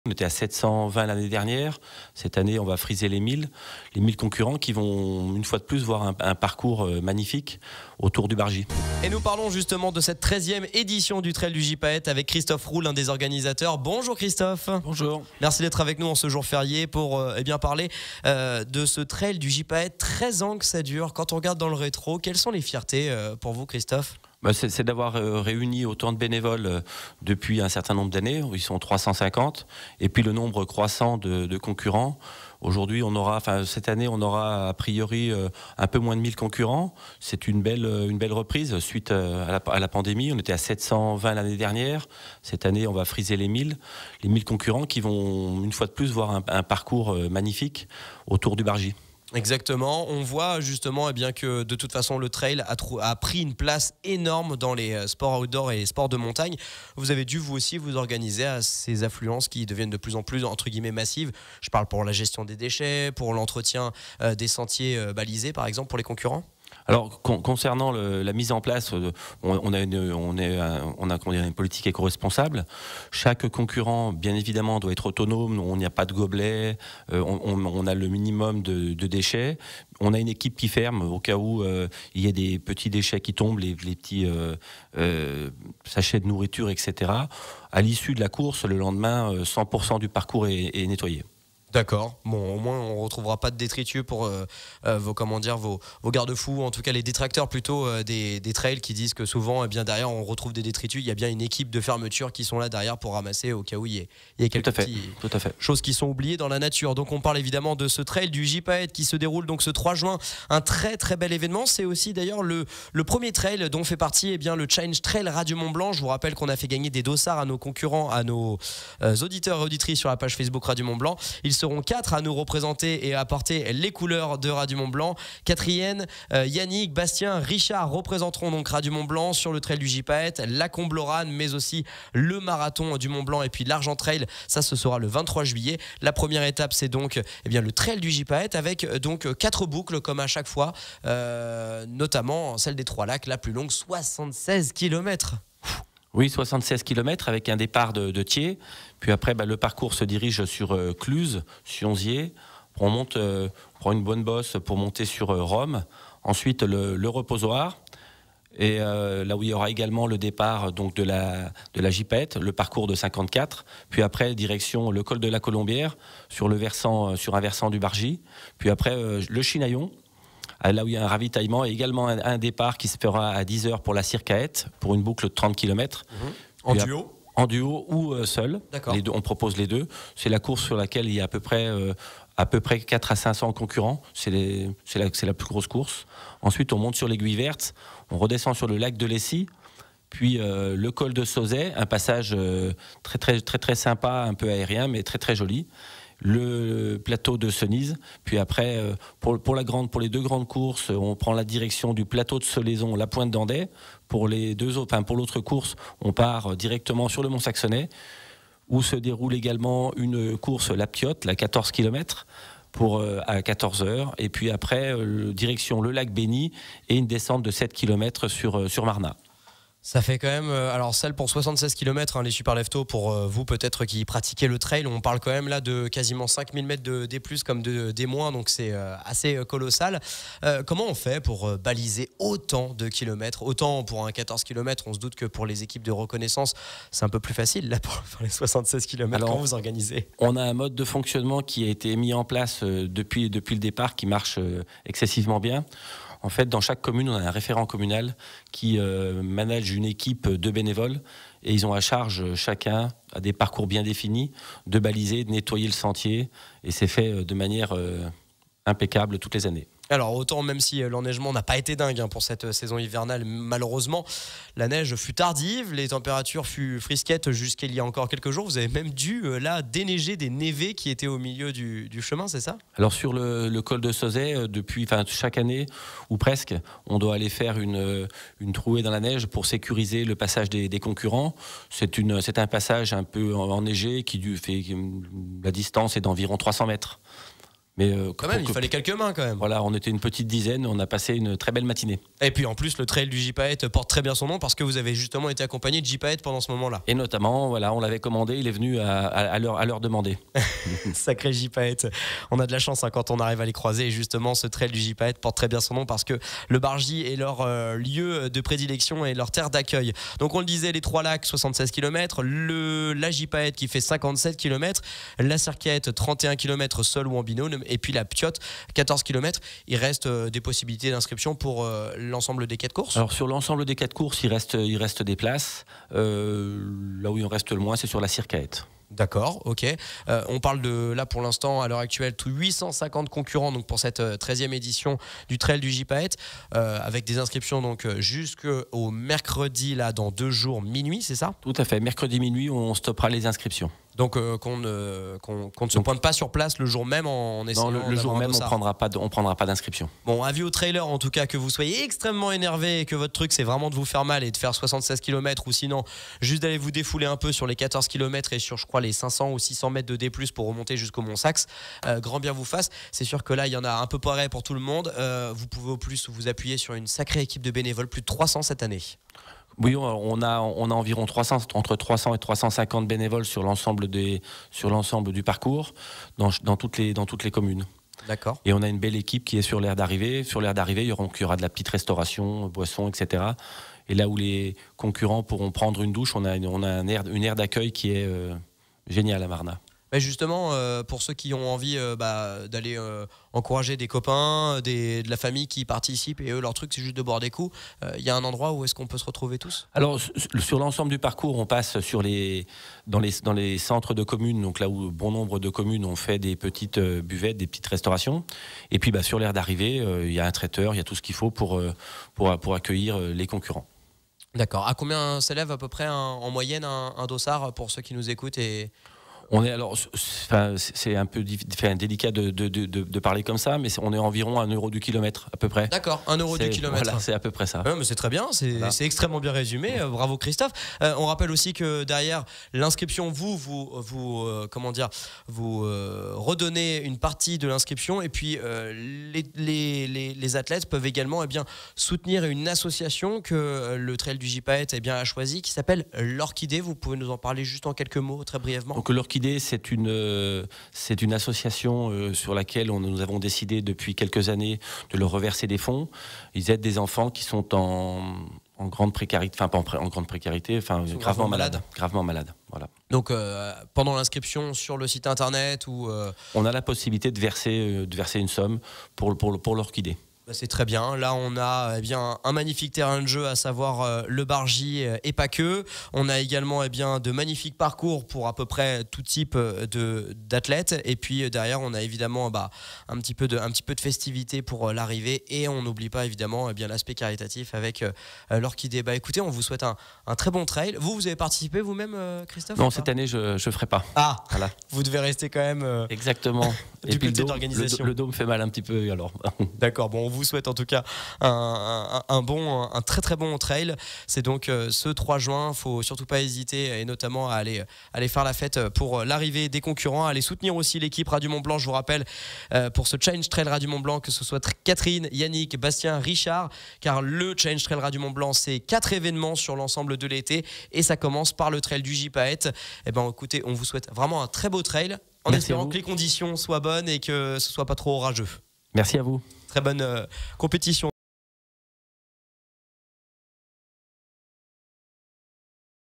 Interview | Christophe Roux - Trail du Gypaète
🎙️ "On était à 720 l'année dernière, cette année on va friser les 1000 concurrents qui vont voir encore une fois un parcours magnifique au coeur du Bargy."
Christophe Roux était notre invité pour nous parler du Trail du Gypaète, première étape du ChallengeTrail Radio Mont Blanc.